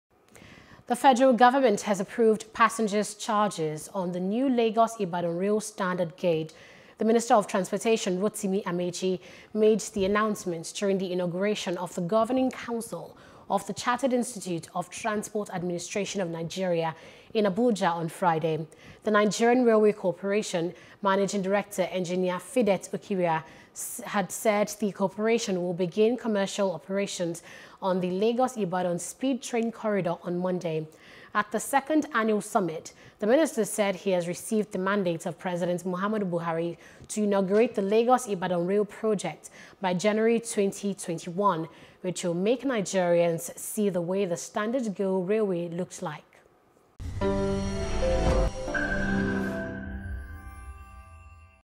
Lagos-Ibadan Railway To Commence Jan. 2021, Fair Charges Btw 3,000 To N6,000 (News | Nigeria)
The Federal Government has approved passengers’ charges on the new Lagos Ibadan rail standard gauge.